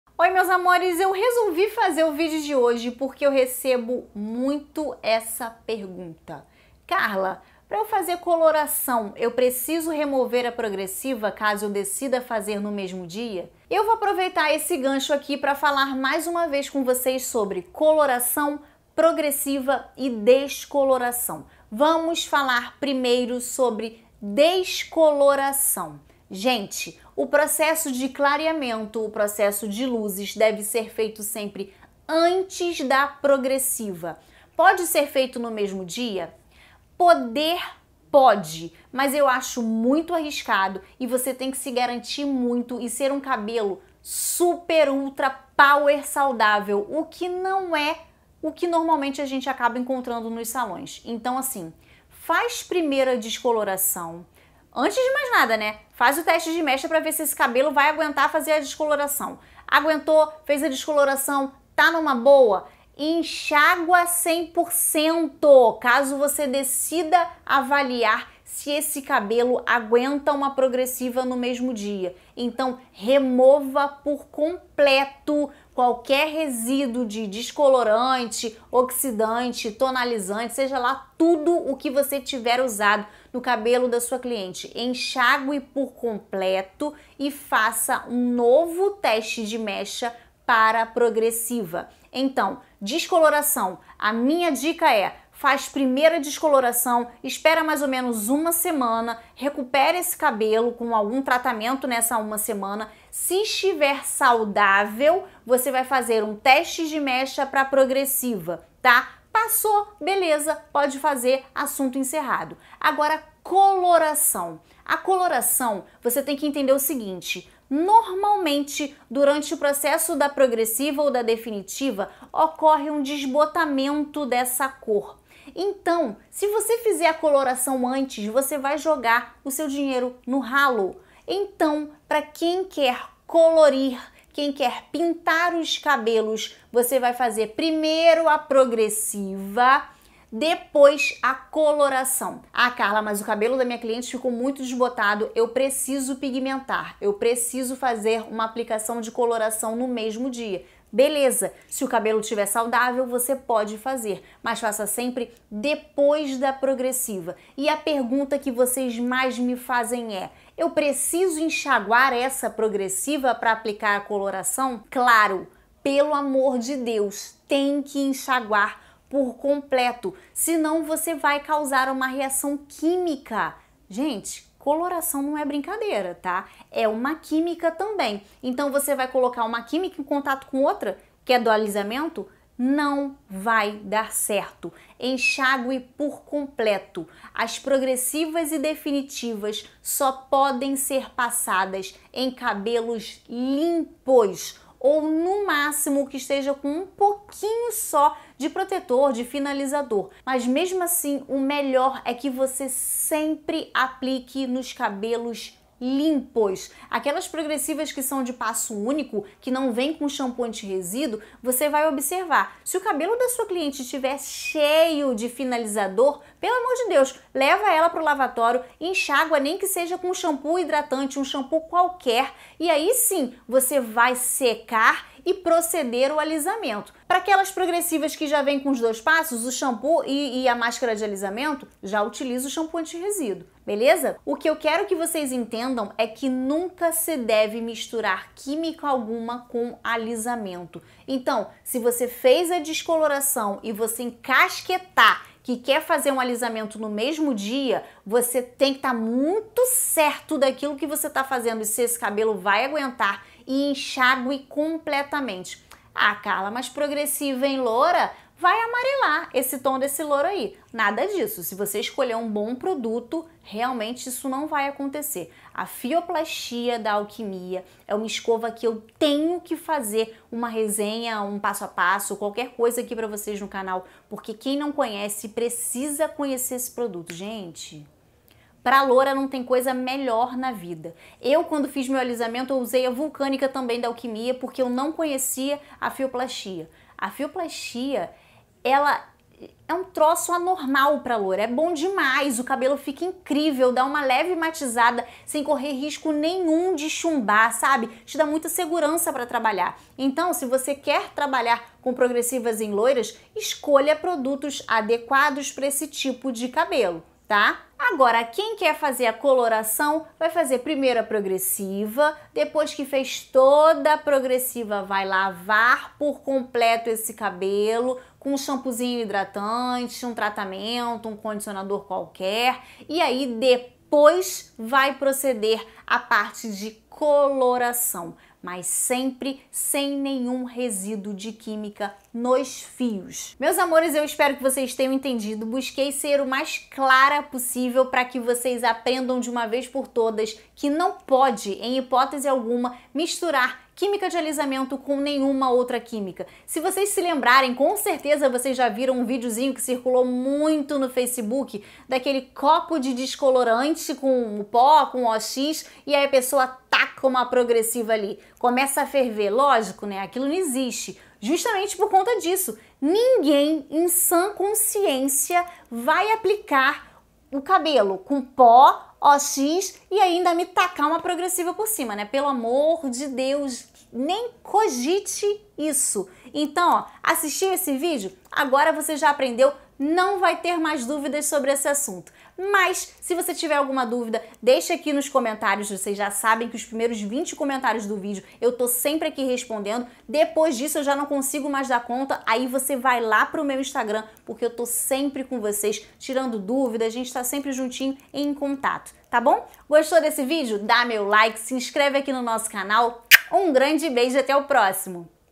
PROGRESSIVA X COLORAÇÃO X DESCOLORAÇÃO
Nesse vídeo ensino quando devemos aplicar a coloração e descoloração com os alisamentos.